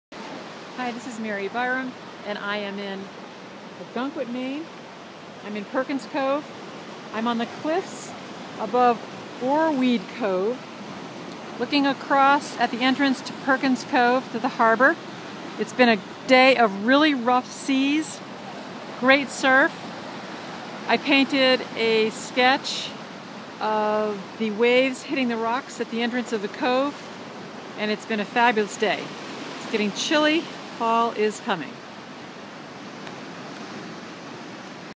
Big Surf Perkins Cove Ogunquit, Maine
Painting in plein air in Perkins Cove on the cliffs above the ocean.